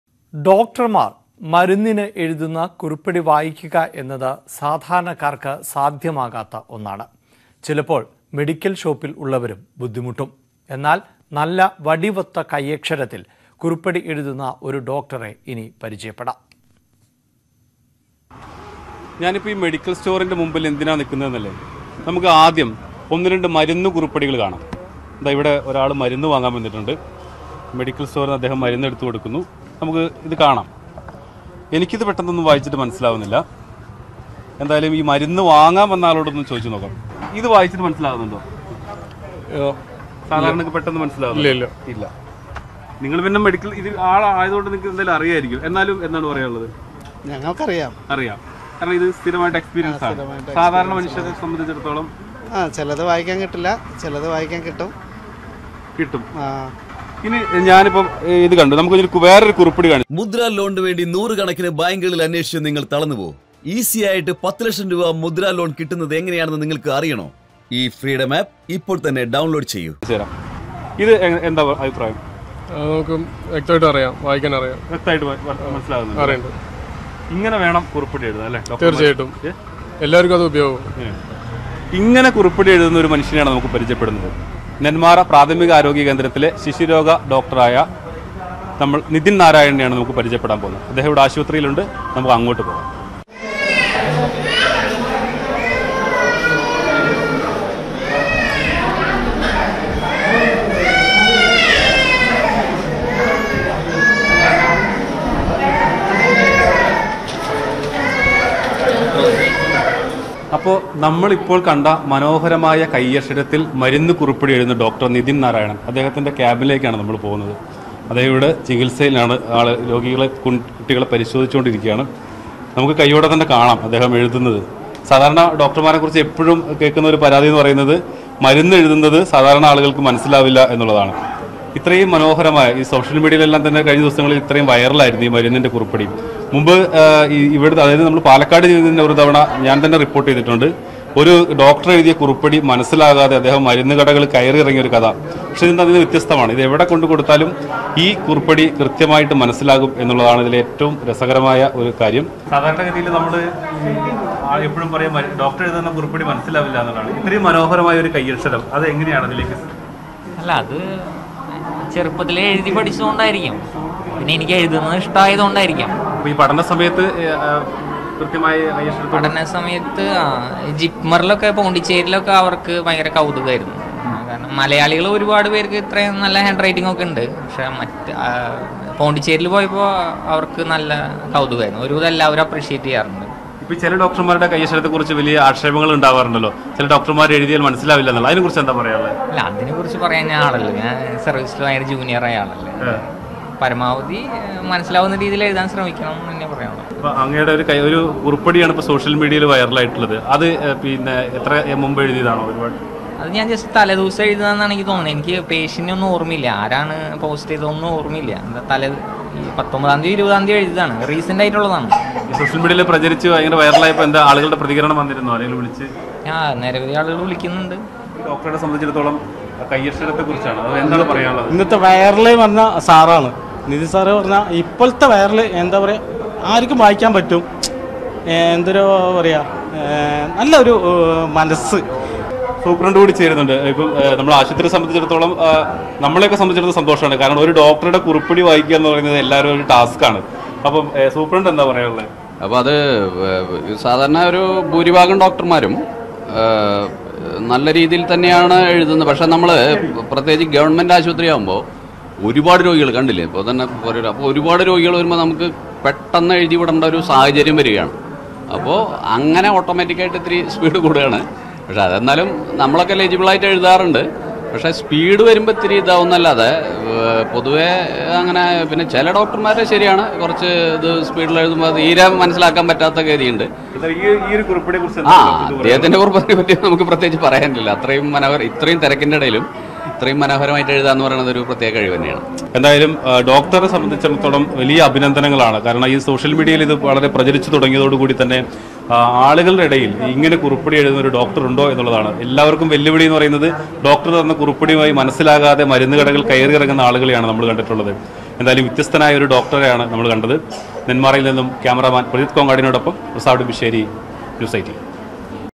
വടിവൊത്ത കയ്യക്ഷരത്തിൽ കുറിപ്പടി എഴുതി വൈറലായിരിക്കുകയാണ് Palakkadലെ ഡോക്ടർ | Kerala News
വടിവൊത്ത കയ്യക്ഷരത്തിൽ മരുന്ന് കുറിപ്പടി എഴുതി വൈറലായിരിക്കുകയാണ് Palakkadലെ ഡോക്ടർ. മറ്റ് ഡോക്ടർമാരിൽ നിന്നും വ്യത്യസ്തമായ എഴുത്തിലൂടെ ശ്രദ്ധ നേടിയിരിക്കുകയാണ് ഈ ഡോക്ടർ.